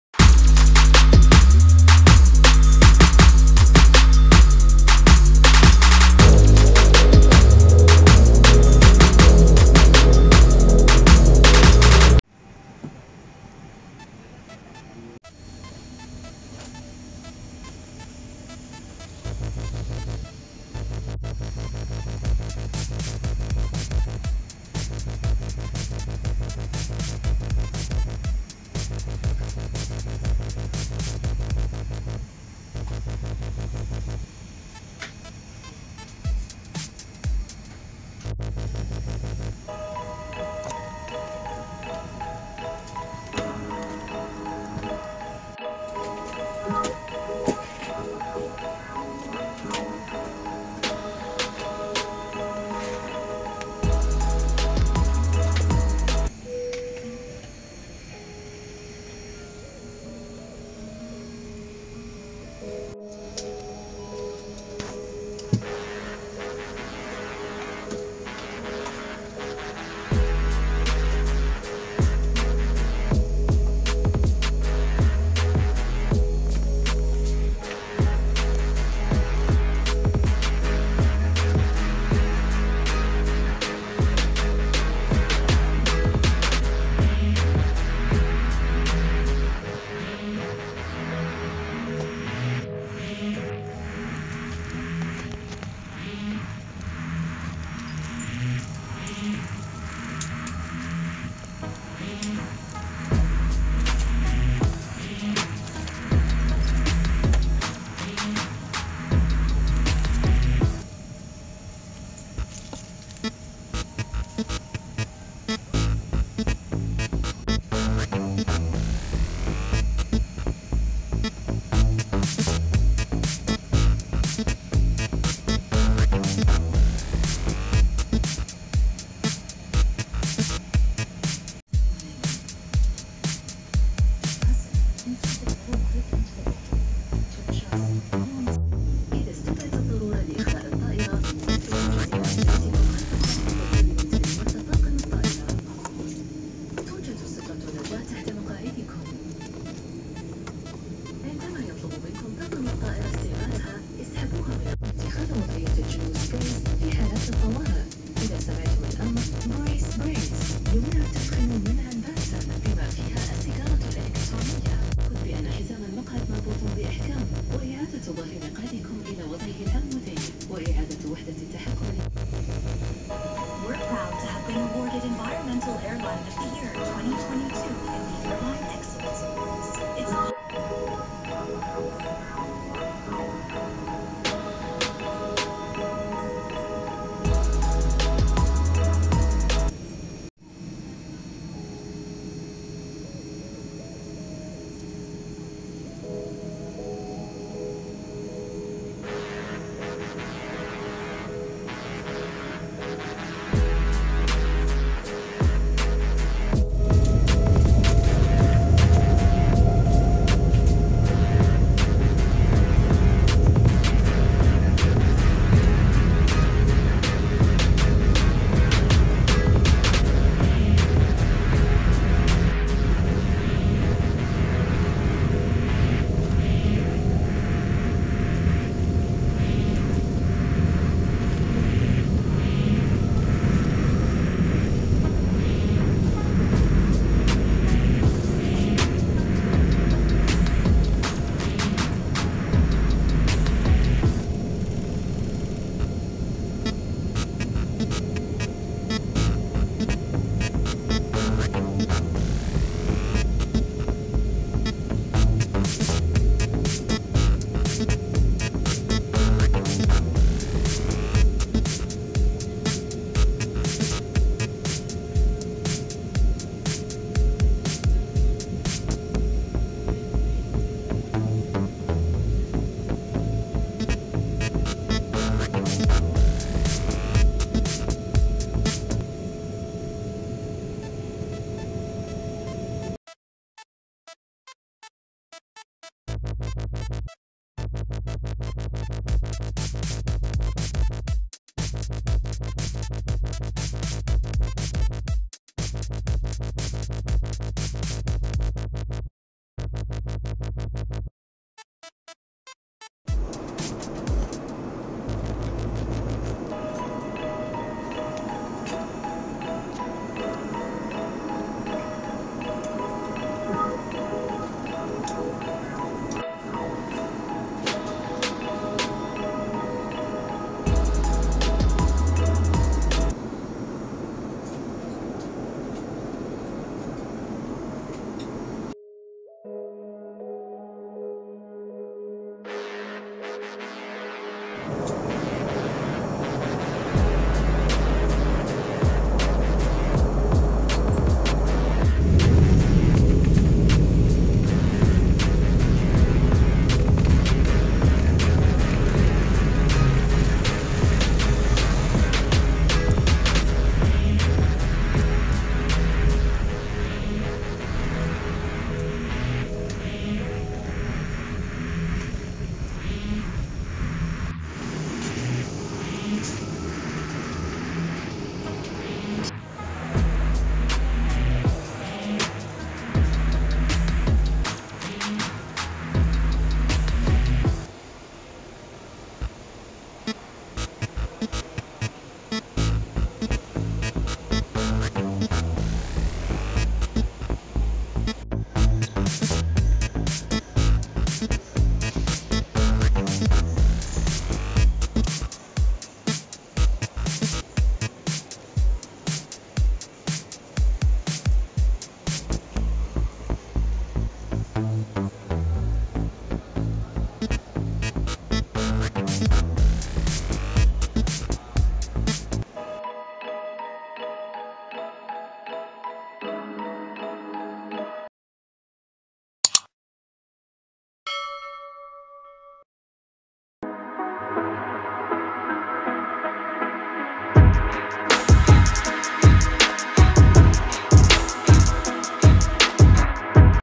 Last Minute Upgraded to Business Class,Chennai to Abu Dhabi - ETIHAD Airways || VL- 21 || We & Data
Last Minute Upgrade from Economy to Business Class, Chennai to Abu Dhabi - ETIHAD  Airways. Business Class experience. 1st international Experience with Business class